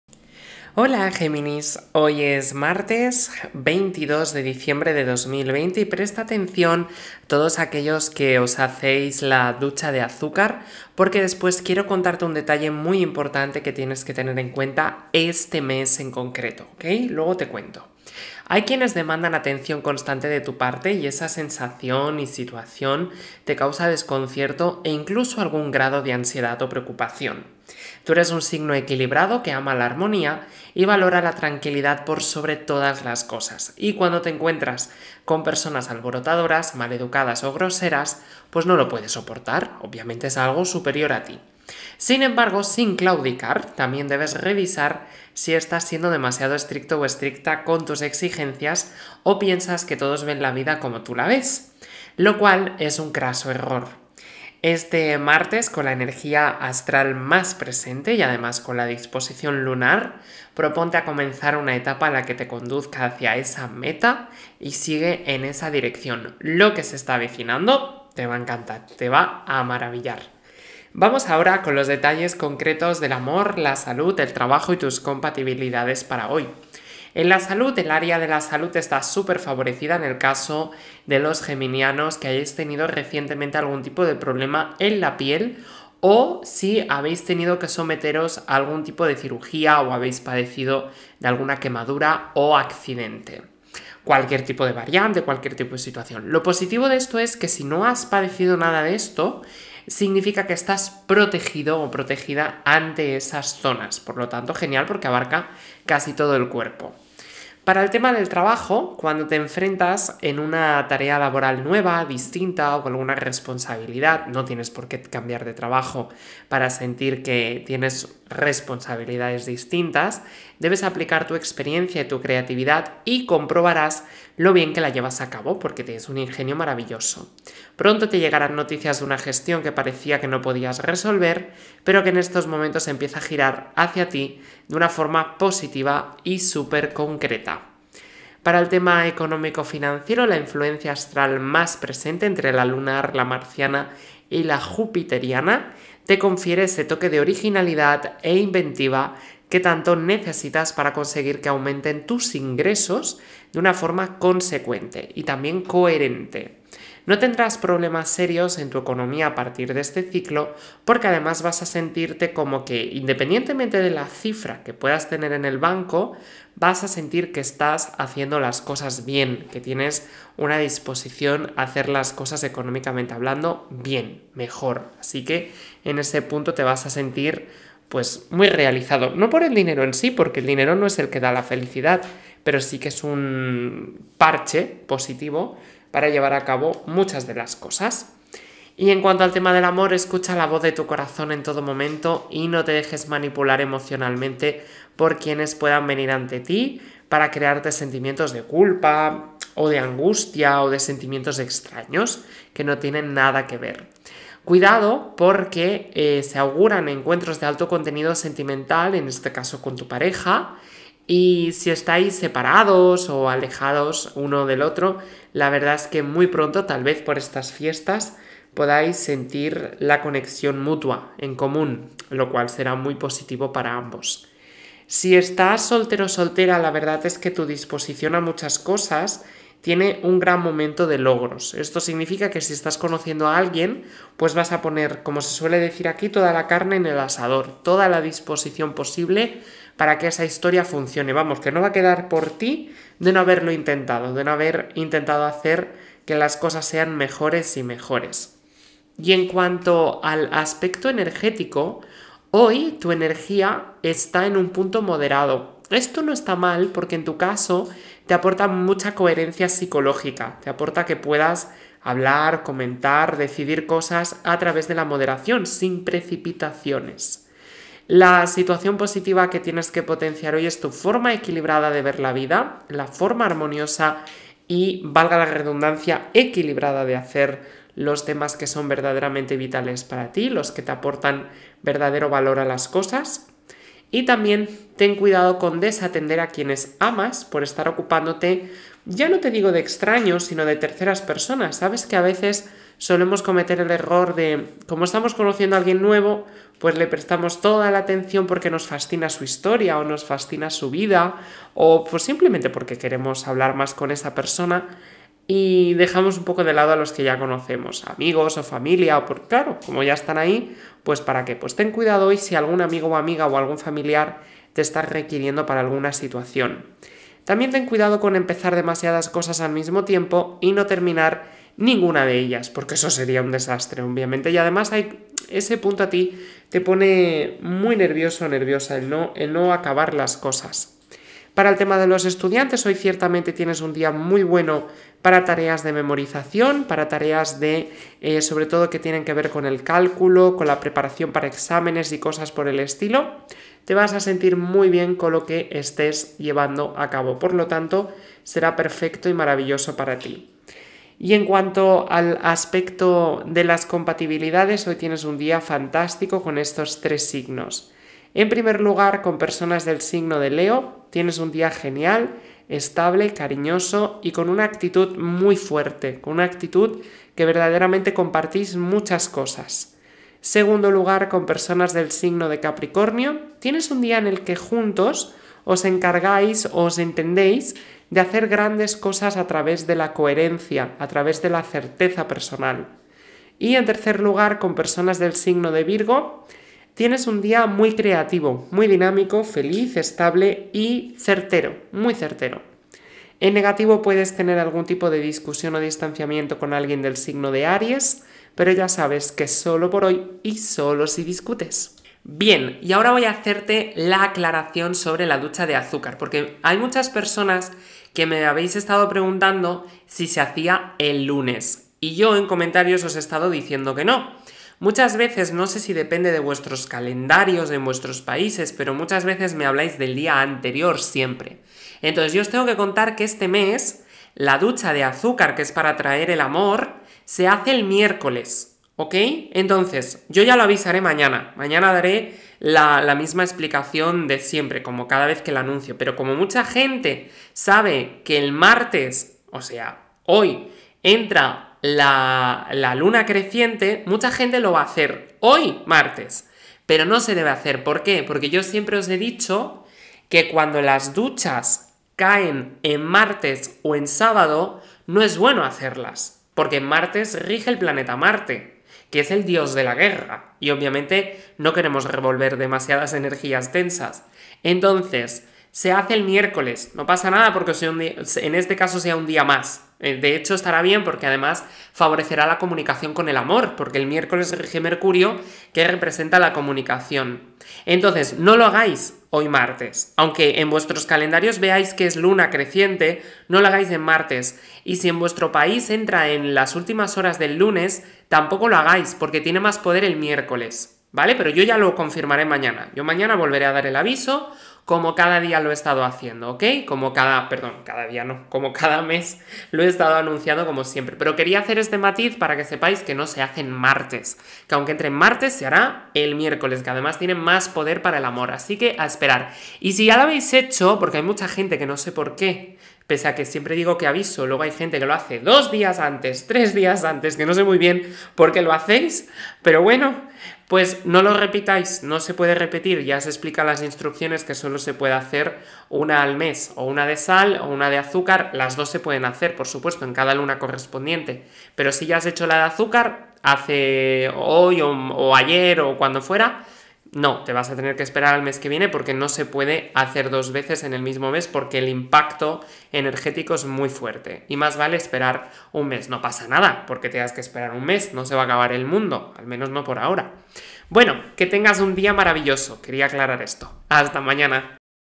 Horóscopo Diario - Géminis - 22 de Diciembre de 2020.
Horóscopo de hoy geminis 22 de Diciembre de 2020. Horóscopo de geminis hoy. Descubre tu futuro con mi horóscopo diario para ti, mi querido signo geminis en el amor, el trabajo, la salud, el dinero y las compatibilidades con otros signos. Horóscopo diario gratis!